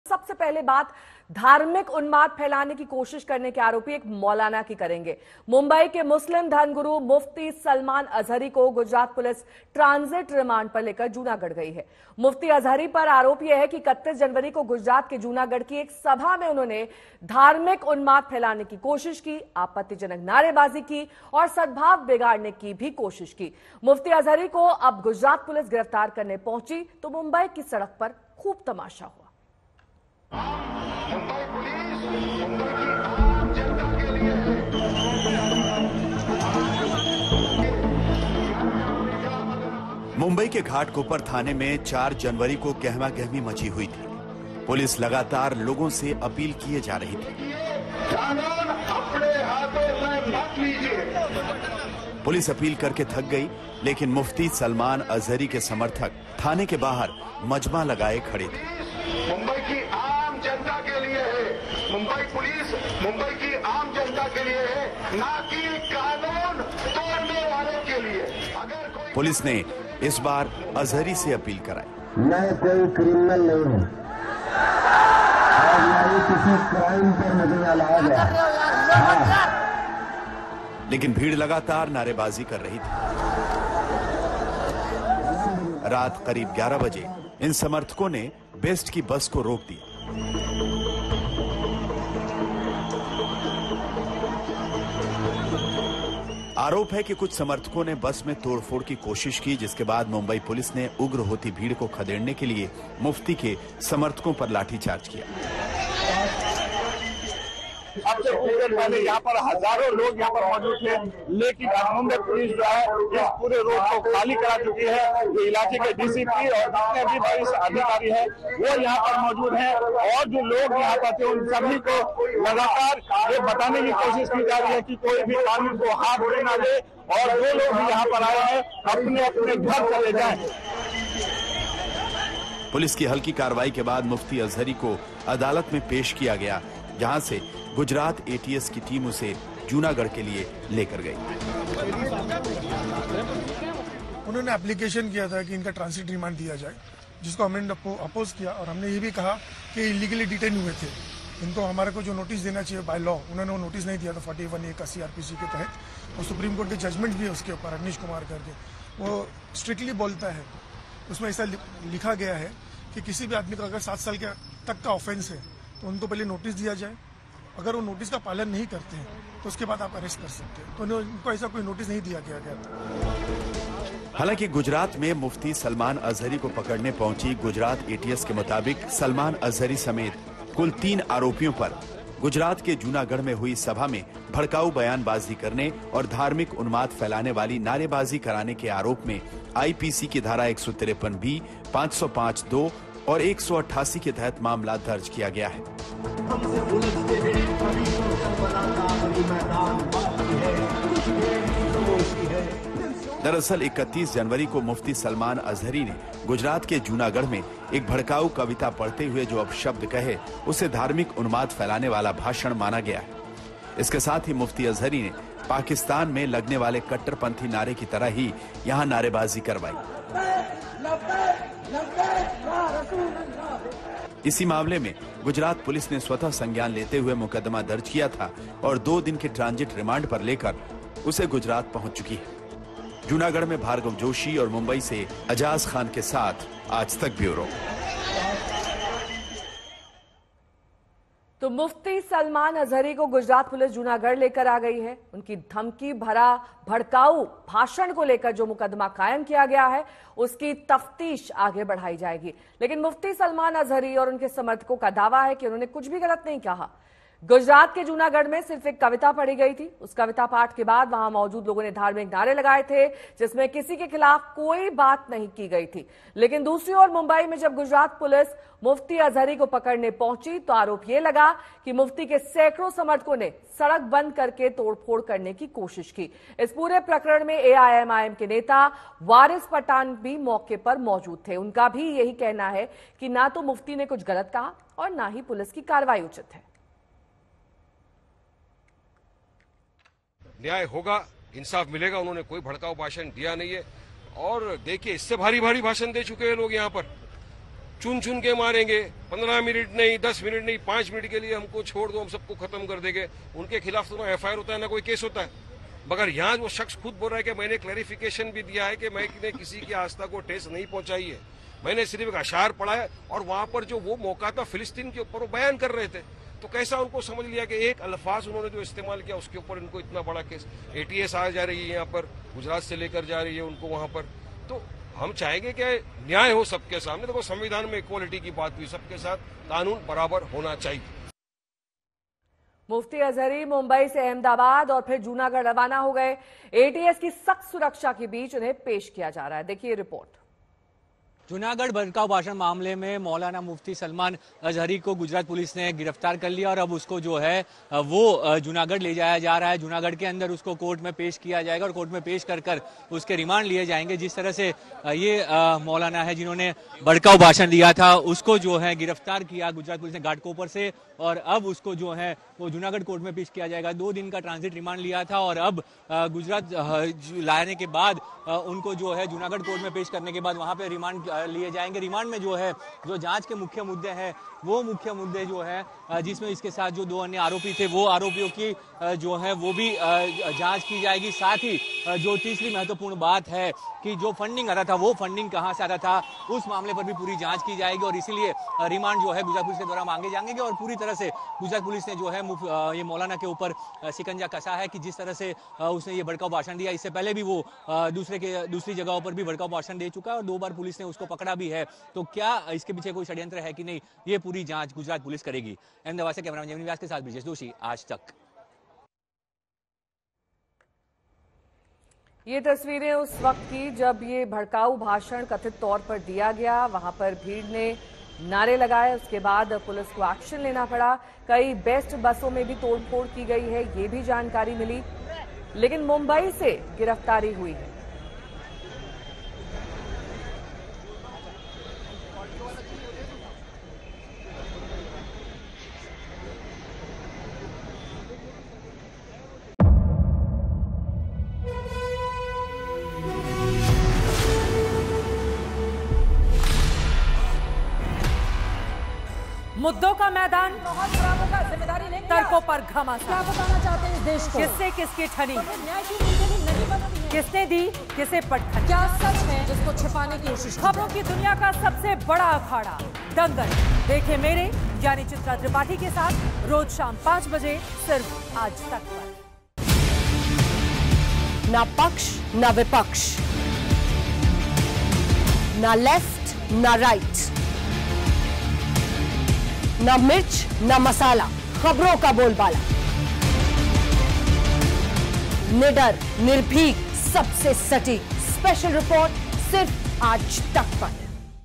Hate Speech Case: मुस्लिम धर्मगुरु मुफ्ती सलमान अजहरी को Gujarat Police ने ट्रांजिट रिमांड पर लिया
मुंबई के मुस्लिम धर्मगुरु मुफ्ती सलमान अजहरी को गुजरात पुलिस ट्रांजिट रिमांड पर लेकर जूनागढ़ गई है. मुफ्ती अजहरी पर आरोप है कि 31 जनवरी को गुजरात के जूनागढ़ की एक सभा में उन्होंने धार्मिक उन्माद फैलाने की कोशिश की...आपत्तिजनक नारेबाजी की और सद्भाव बिगाड़ने की कोशिश की. मुफ्ती अजहरी को जब गुजरात पुलिस गिरफ्तार करने पहुंची तो मुंबई में सड़क पर खूब तमाशा हुआ.